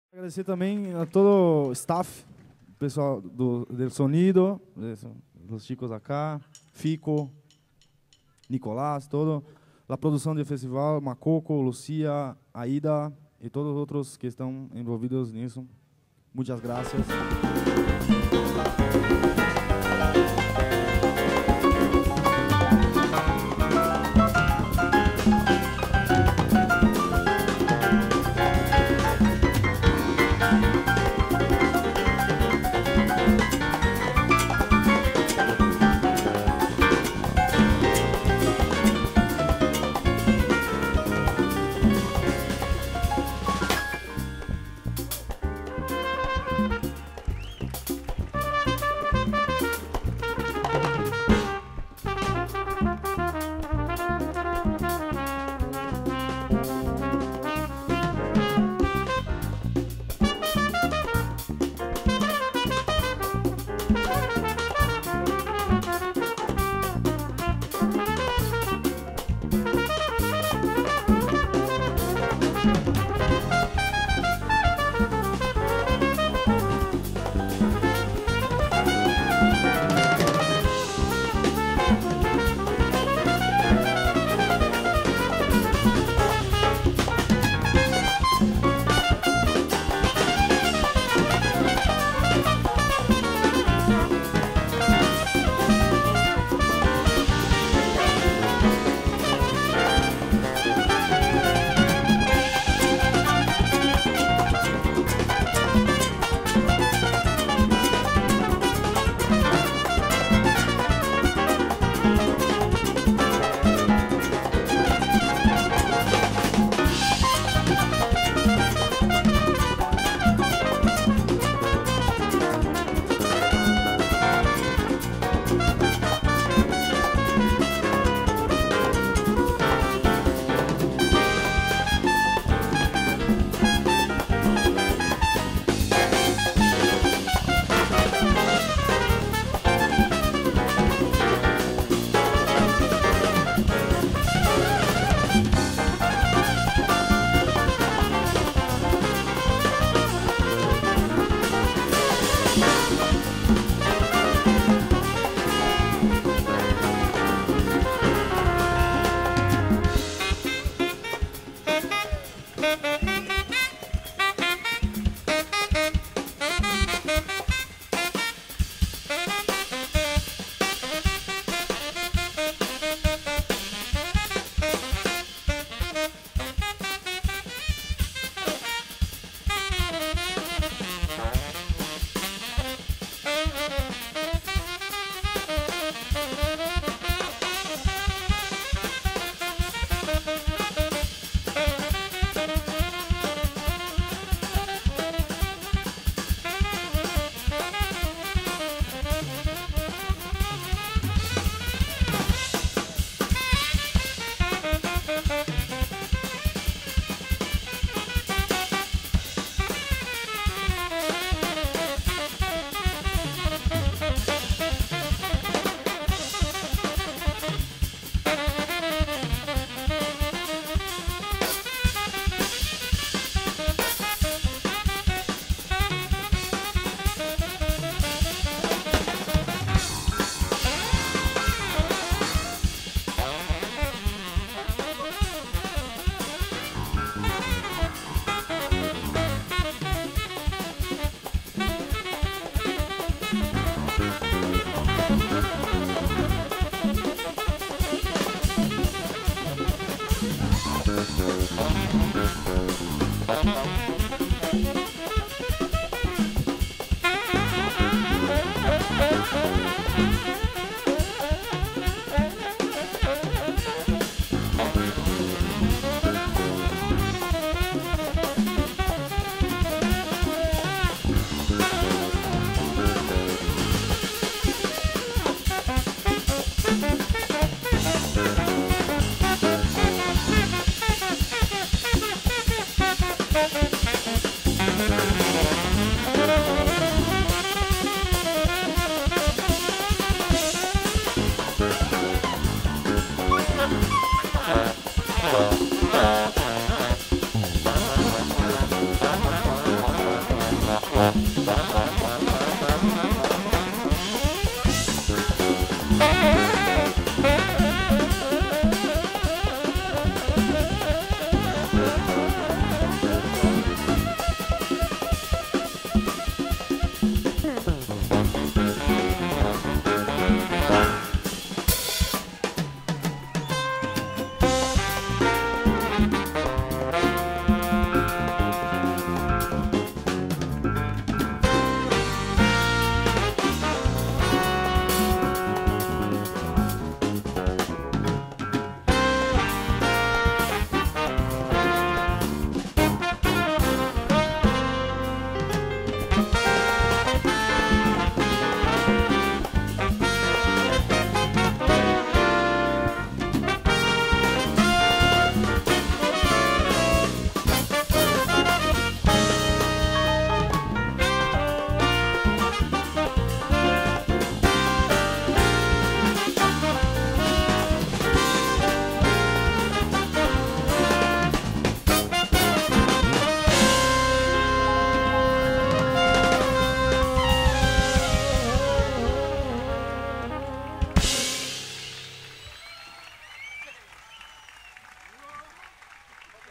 Jazz a la Calle 2014 - Afro Latin Jazz
Fragmento de Afro Latin Jazz, de Brasil, en el marco del 8 Encuentro de Músicos Jazz a la Calle. Mercedes, Uruguay, enero 2014. Realizacion de 4zetas contenidos

Cámaras de Majo Bonilla, Alexis Moranti, Clara Esmoris, Celeste Rojas y Diego Lozza